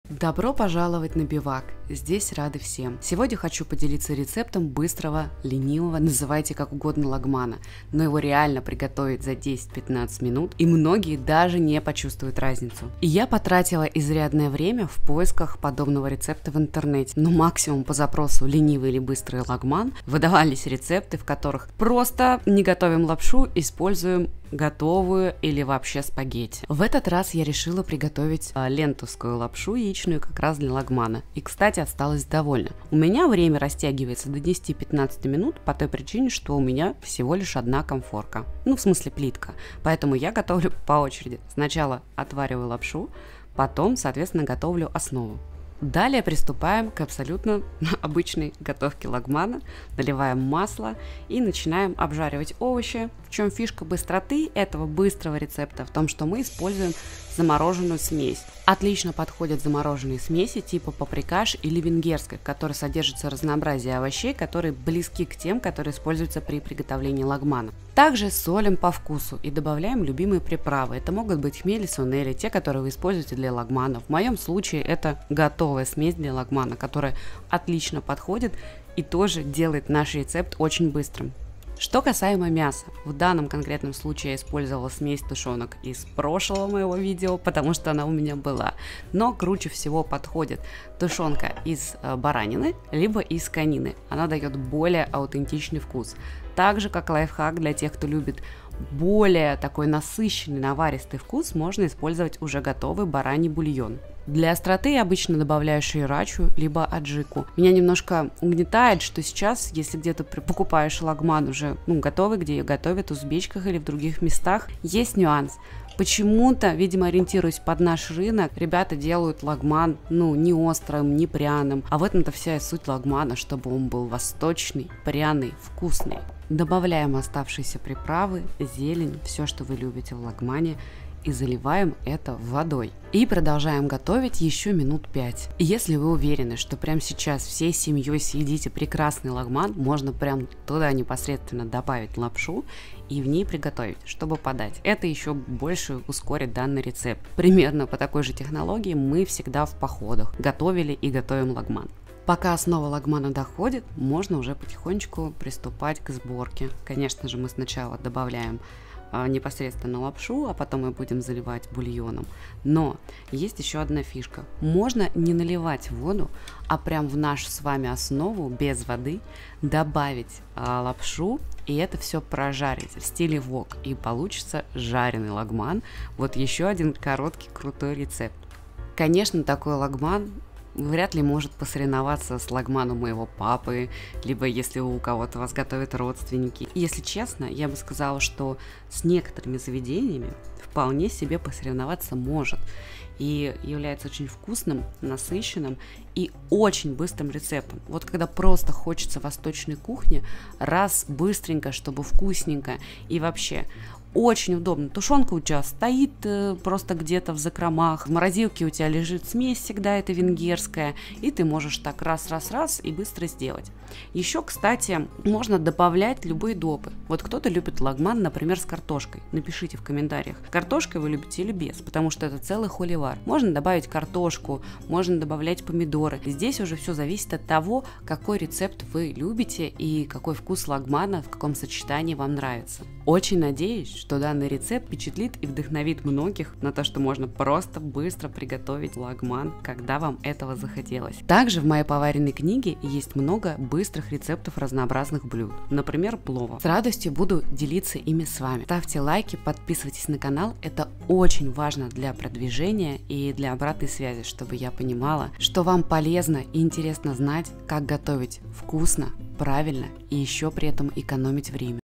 Самый БЫСТРЫЙ ЛАГМАН во вселенной | ленивый лагман рецепт
Как приготовить быстро и вкусно лагман? У нас есть ответ на этот вопрос!
Рецепт лагмана за 10 минут:
1. Готовая яичная лапша для лагмана
2. Замороженная смесь (Венгерская, Лечо, Паприкаш)
3. Тушенка (Баранина, Конина)
4. Вода примерно 1л., специи (опционально готовый бульон из баранины)